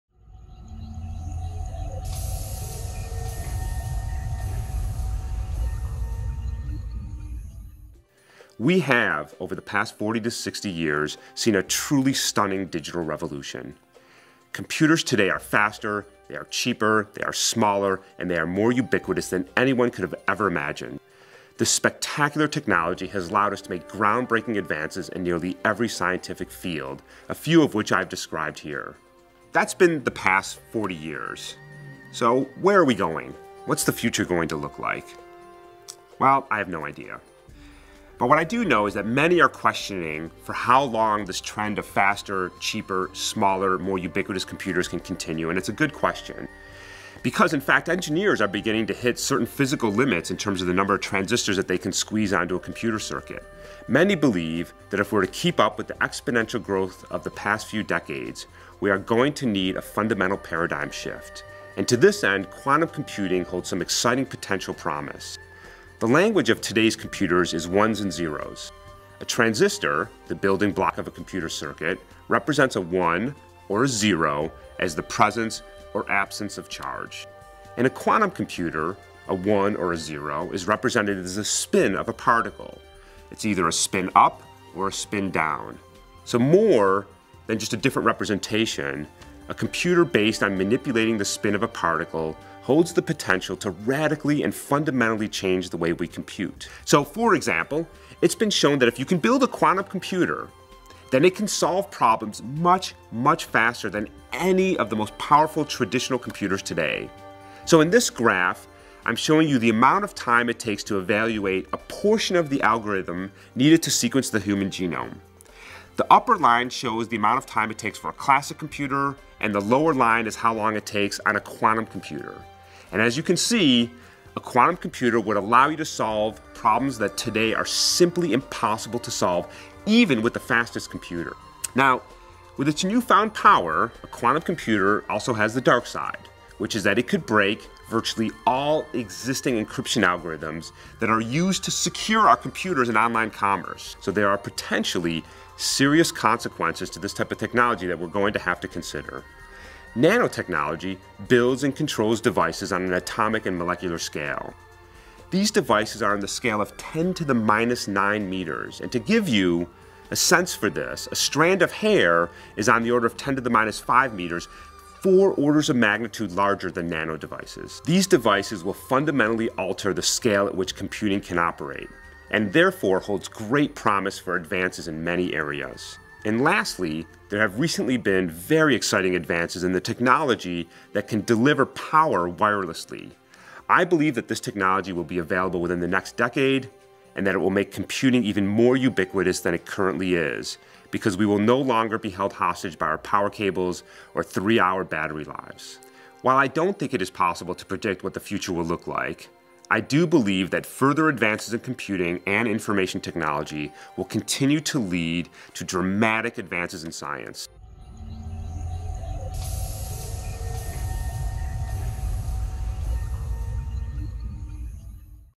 The Future of Computing
Hany Farid, Professor of Computer Science and Director of the Neukom Institute for Computational Science, provides a brief overview of the future of computational science.